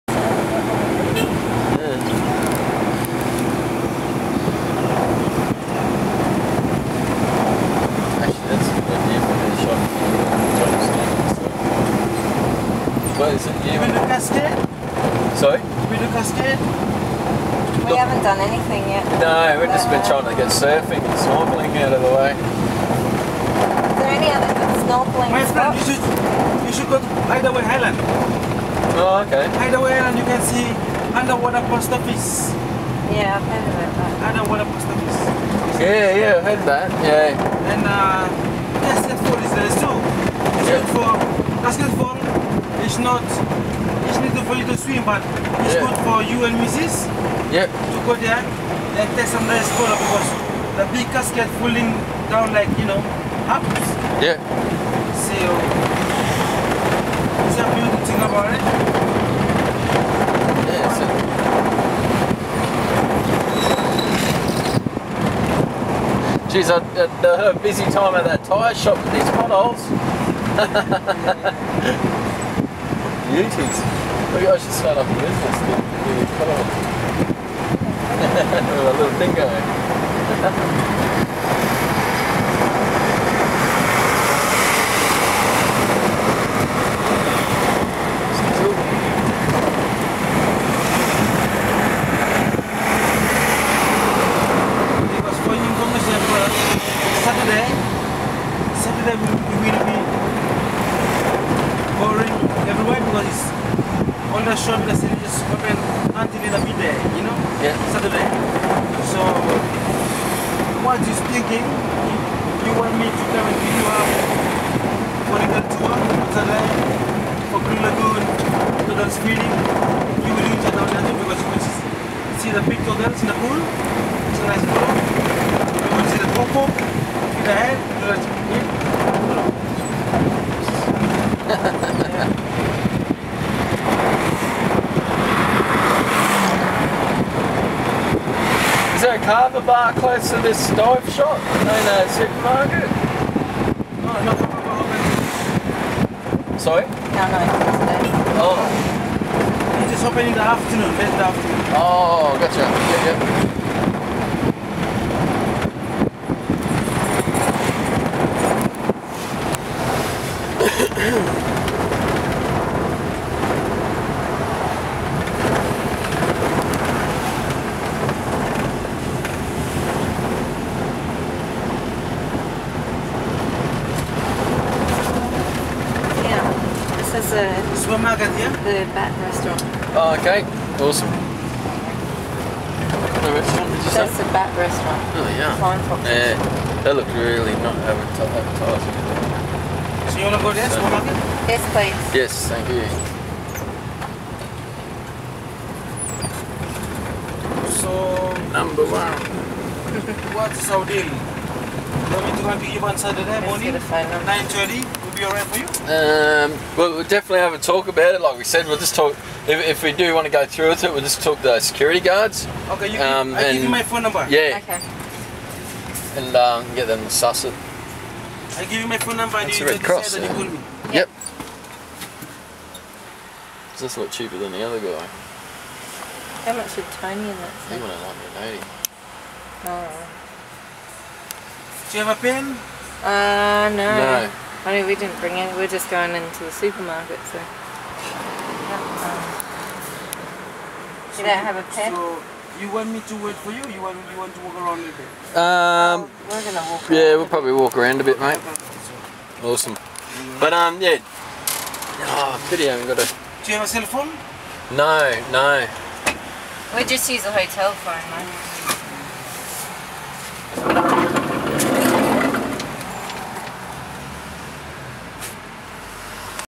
drive to town vanuatu from breakas resort
drive to town in bus vanuatu. bit in air at start sorry  but gets a bit better. another busdriver tour. u shouldmnot bay more than 150 vat each into town flat